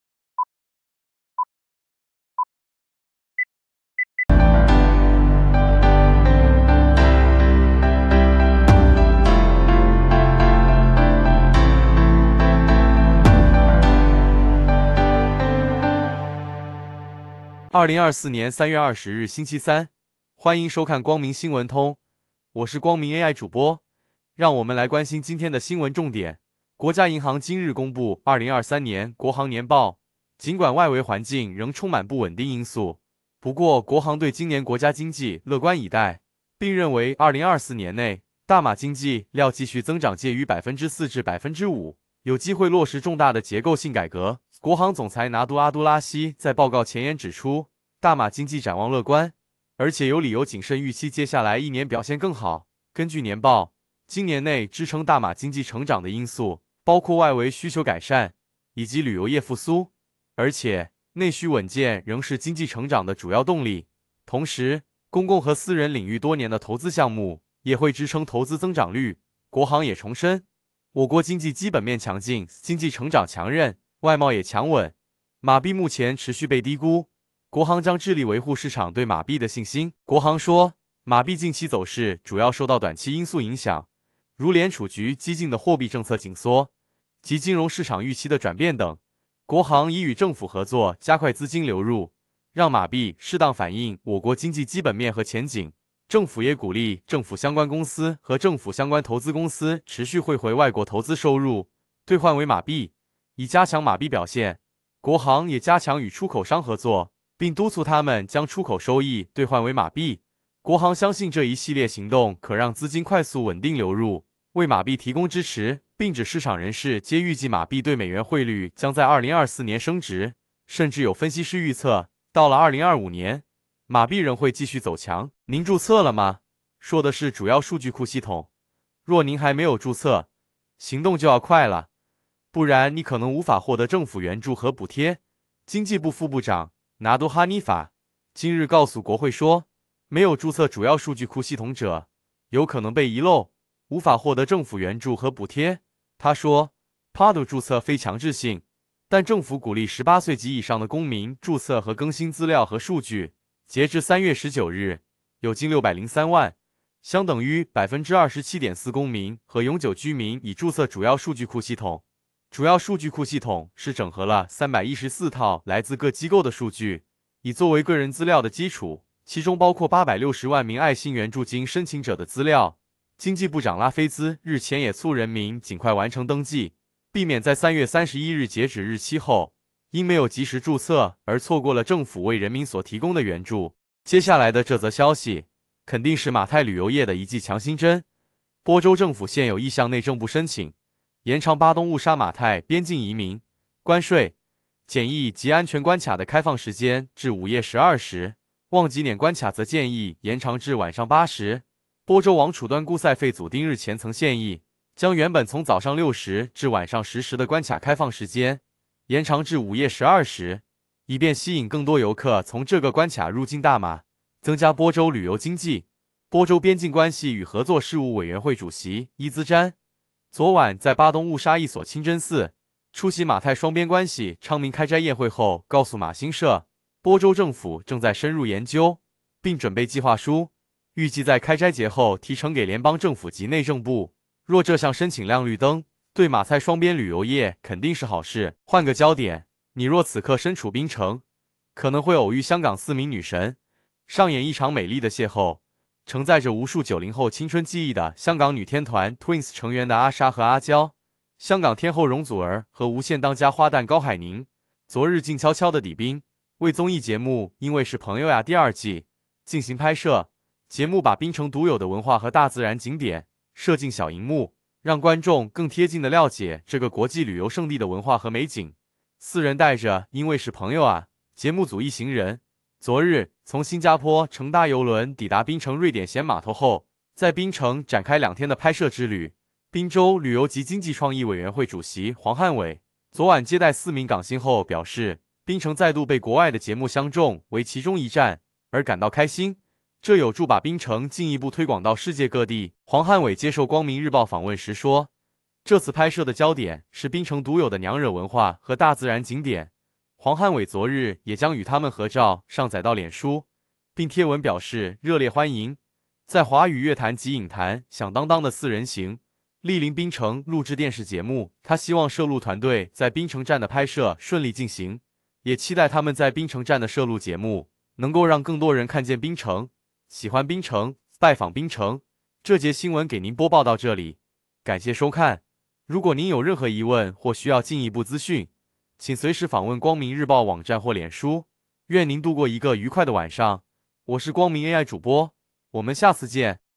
【光明新聞通】2024年3月21日夜報封面焦點
◆【2023國行年報】國內外消費強勁 明年經濟更好
◆沒註冊PADU 或失津貼
◆玻2馬泰關卡 申請延長開放時間
◆因為是朋友呀第二季 Twins容祖兒高海寧檳取景
光明新聞通，讓您掌握新聞時事，一點就通！欲知詳細新聞內容，購閱《光明日報》，點擊 或訂閱光明電子報